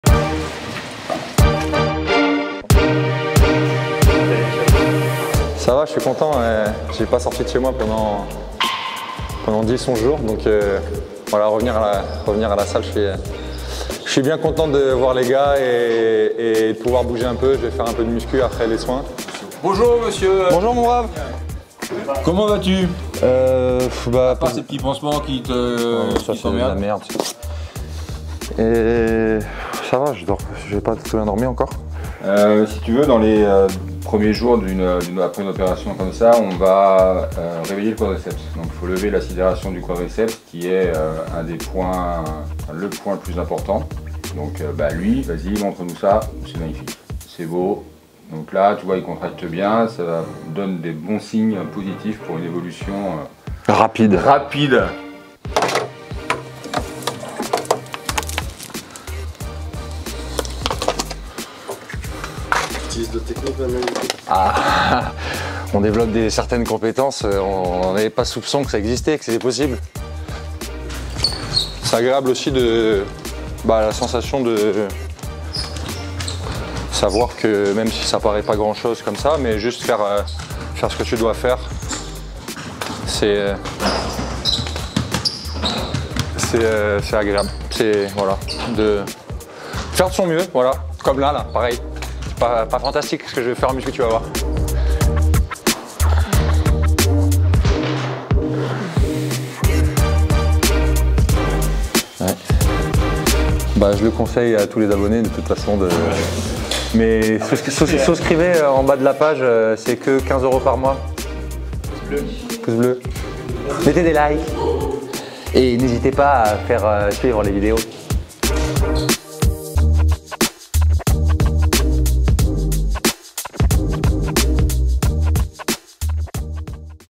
🏟 Joffrey Lauvergne | Step 1 - Retour à l'Astroballe
🔝 Le retour de Joffrey Lauvergne à l'Astroballe ! 🏟

📽 Découvrez les premières images de son rétablissement et ses premières sensations quelques semaines après sa blessure 💪